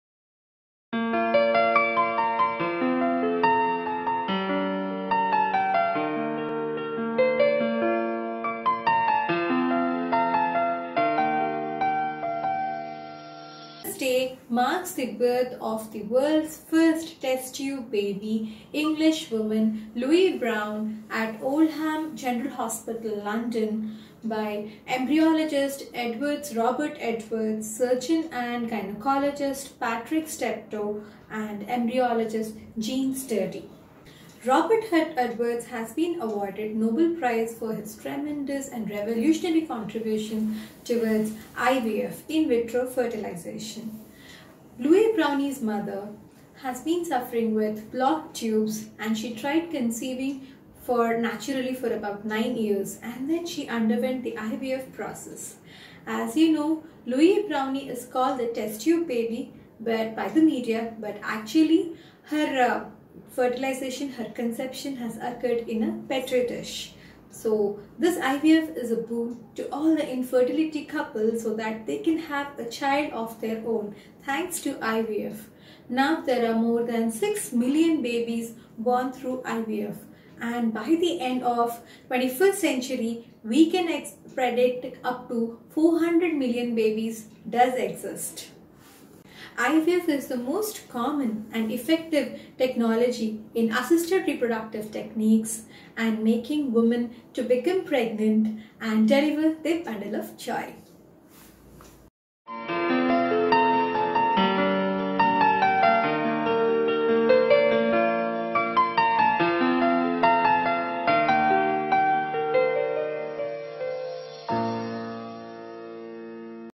How IVF was discovered and more
Dr. Naga Sudha Lakshmi here is telling you all about the evolution of IVF in a very interesting video.